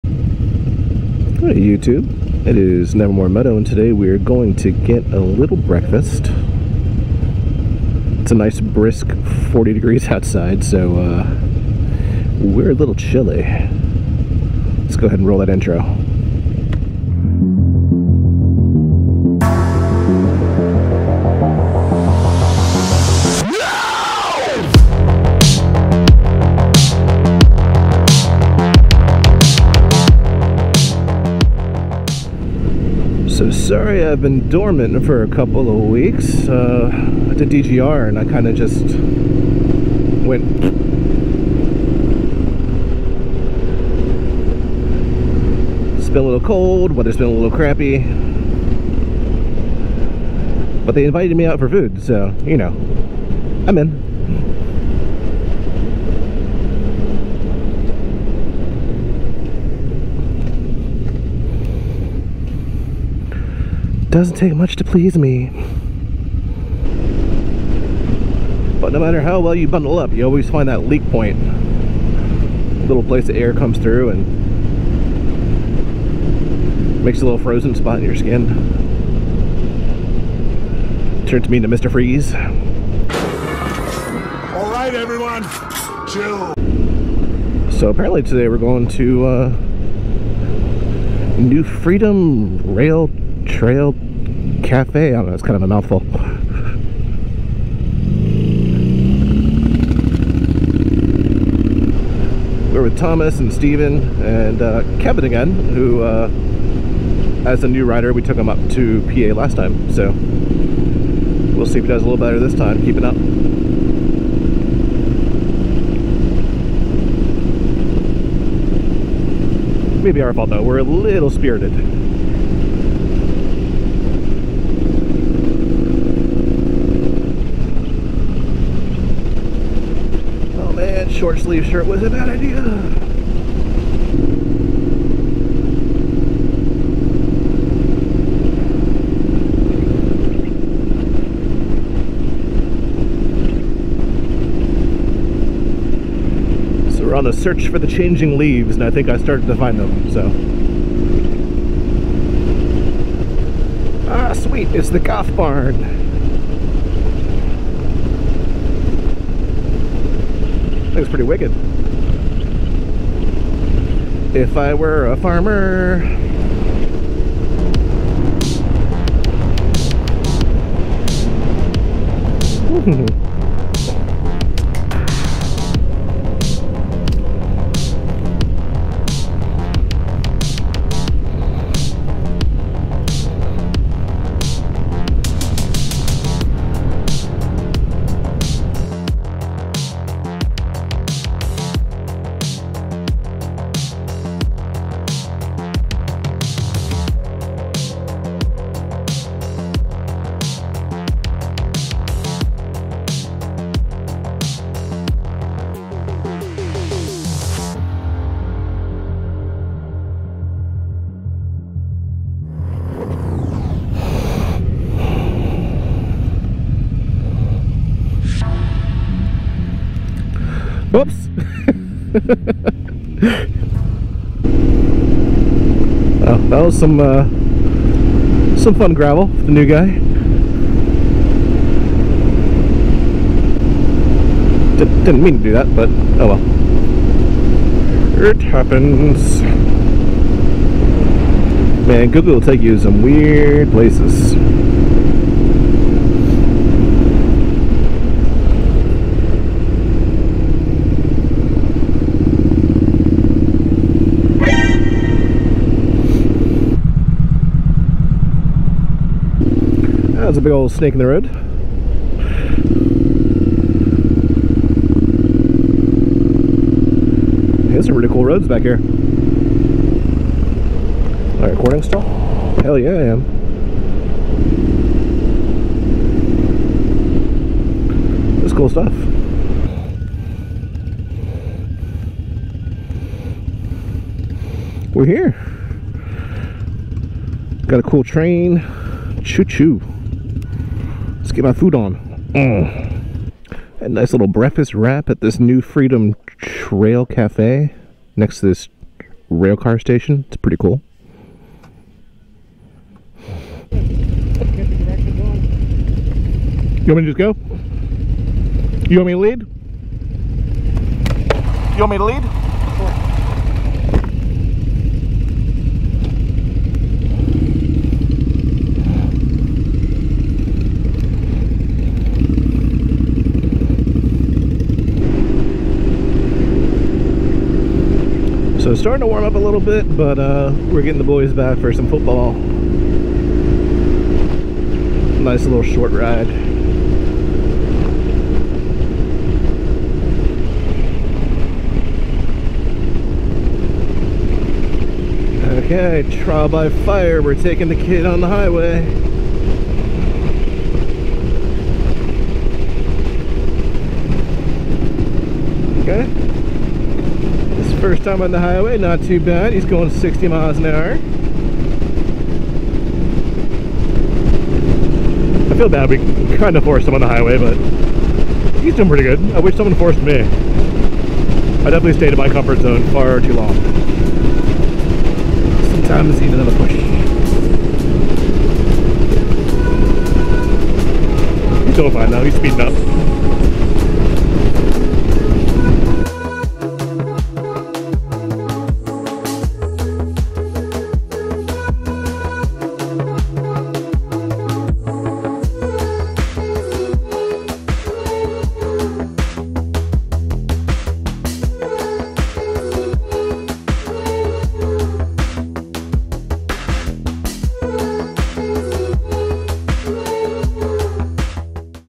Triumph Street Scrambler in the Beautiful Fall Foliage
Just a short ride in the cold with some friends for a quick bite to eat. Enjoying the fall foliage and the company.